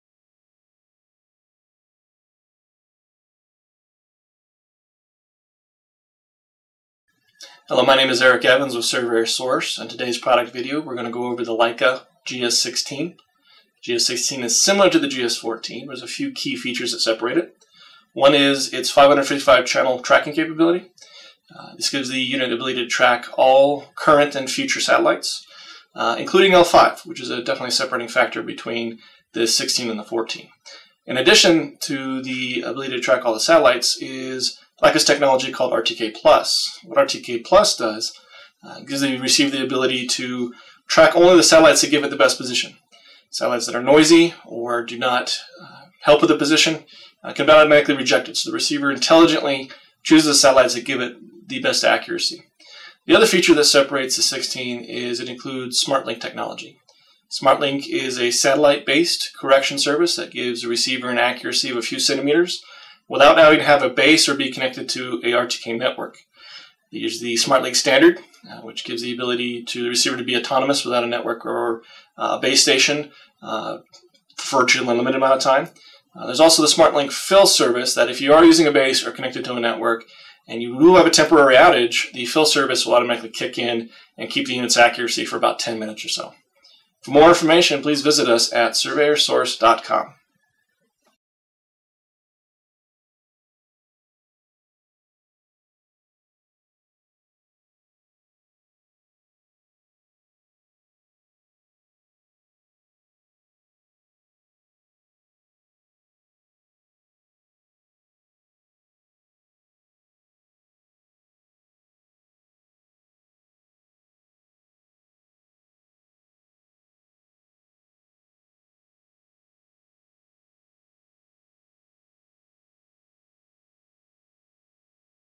Leica Viva GS16 – Self-Learning GNSS Smart Antenna
The Leica Viva GS16 is the most compact and powerful, self-learning GNSS smart antenna. Working with the leading RTK technology, the Viva GS16 meets the highest standards in measurement excellence with RTKplus and SmartLink. The growing number of signals from an ever increasing satellite constellation demands a GNSS receiver to be smarter than ever before. RTKplus intelligently adapts to changing conditions by selecting the optimal signals to deliver the most accurate positions. Work more efficiently and in more challenging environments than you’ve ever worked in before.

Enjoy uninterrupted accurate positioning even when your local correction service is unavailable due to obstructions or lack of cellular coverage with the global SmartLink service. Even when no reference data is available, work fully remotely with SmartLink.